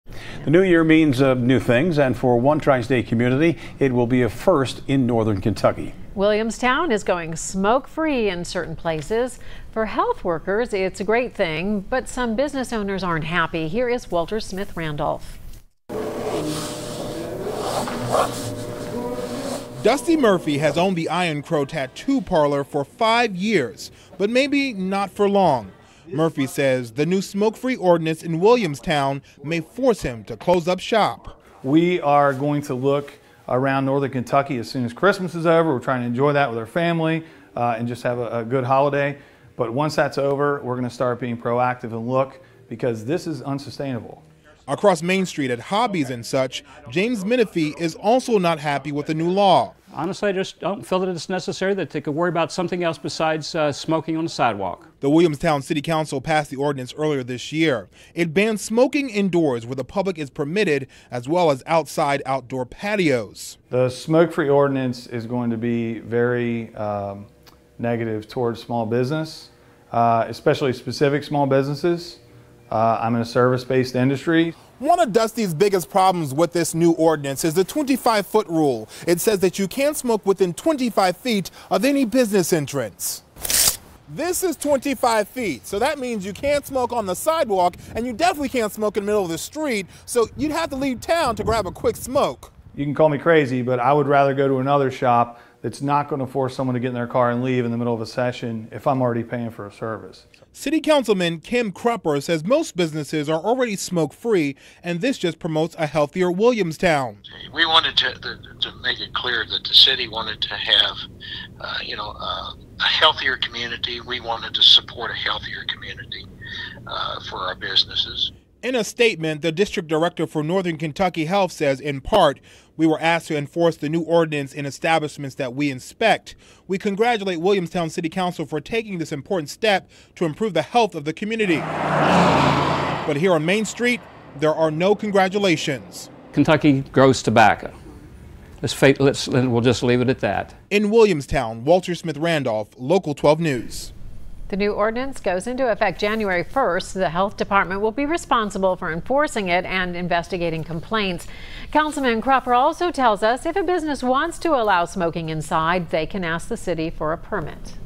Parts of Williamstown going smoke-free in 2019
WILLIAMSTOWN, Ky. (WKRC) - The new year means new things, but for one local community, it'll be a first in northern Kentucky as parts of Williamstown will go smoke-free - and many business owners aren't happy about it.

The Williamstown City Council passed the ordinance earlier in 2018, banning smoking indoors where the public is permitted and outdoor patios.

"The smoke-free ordinance is going to be very negative toward the small businesses," Dusty Murphy said. "Especially specific small businesses. I'm in a service-based industry."

Murphy, owner of the Iron Crow Tattoo Parlor for five years, says the new smoke-free ordinance in Williamstown may force him to close up shop.

"We are going to look around northern Kentucky as soon as Christmas is over," Murphy said. "We're going to try to enjoy that with family, have a good holiday. But once that's over, we're going to start being proactive and look becasue this is unsustainable."

Across Main Street at Hobbies and Such, James Menneffee is also unhappy with the new law.

"Honestly, I don't feel like it's necessary," Menneffe said. "They can worry about something else besides smoking on the sidewalk."

One of Murphy's biggest concerns about the ordinance is the 25' rule, meaning you cannot smoke with 25' of an entrance to a business.

"You can call me crazy but I would rather go to another shop that's not going to force somebody to get in their car and leave in the middle of a session if I'm already paying for a session," Murphy said.

Williamstown City Councilwoman Kim Crupper says most businesses are already smoke-free and this just promotes a healthier Williamstown.

"We wanted to make it clear that the city wanted to have a healthier community," Crupper said. "We wanted to support a healthier community for our businesses."

The district director for Northern Kentucky Health says in part "we were asked to enforce the new ordinance in establishments that we inspect. We congratulate Williamstown City Council for taking this important step to improve the health of the community."

The new ordinance goes into effect on Jan. 1.